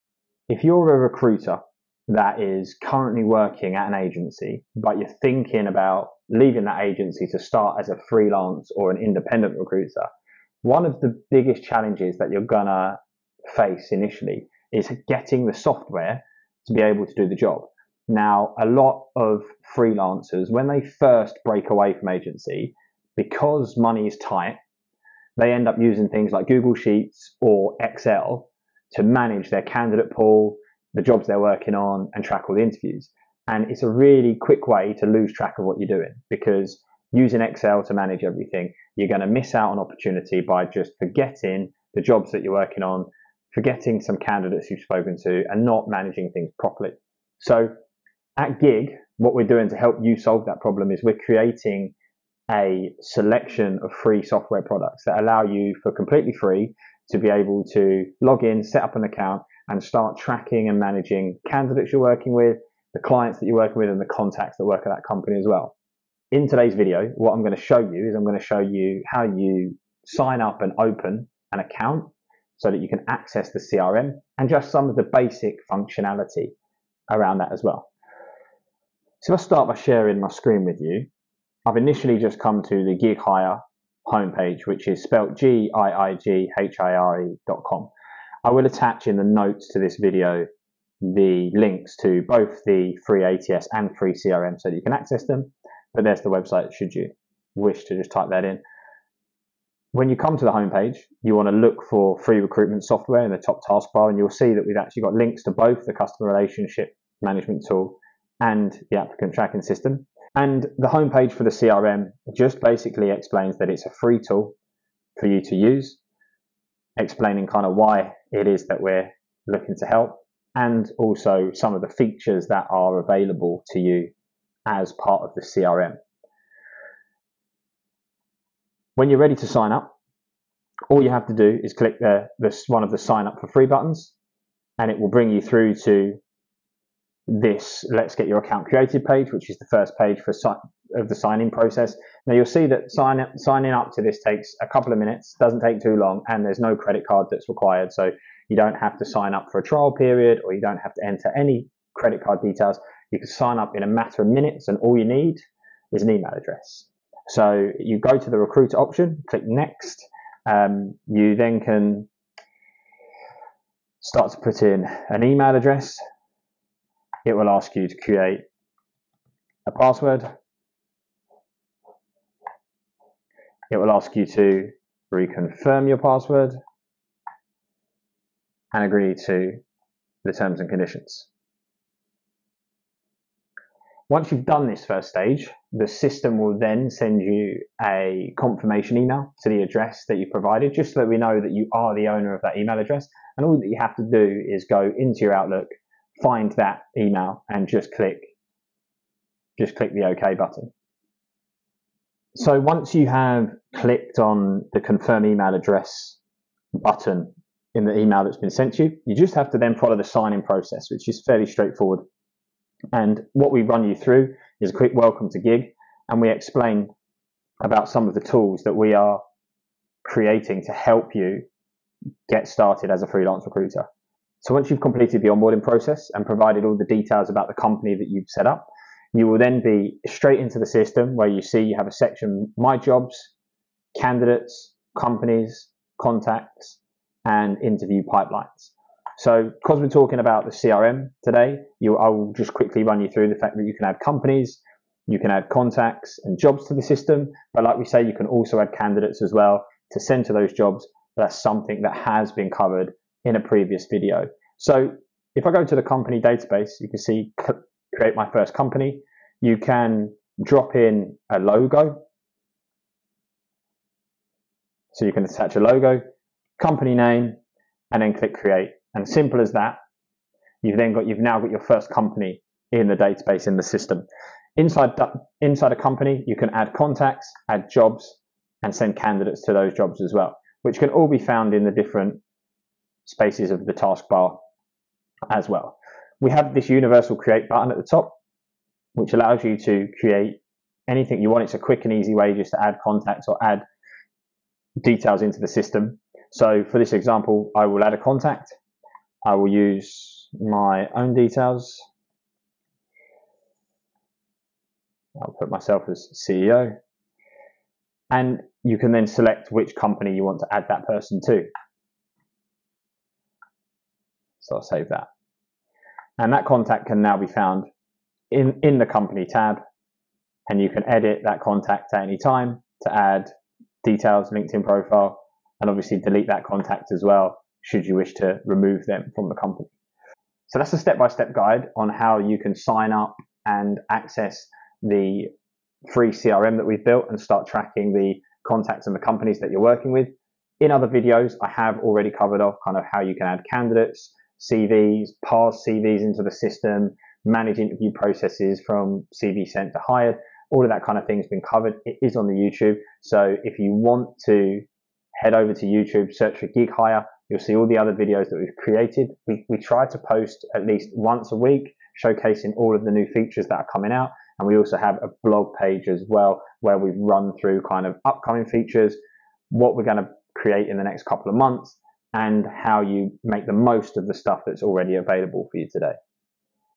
How To Open a Free Recruitment CRM - Free Recruitment Software
In this video, I cover off how you can access a free recruitment CRM to manage the companies and contacts you're working with, without spending a penny!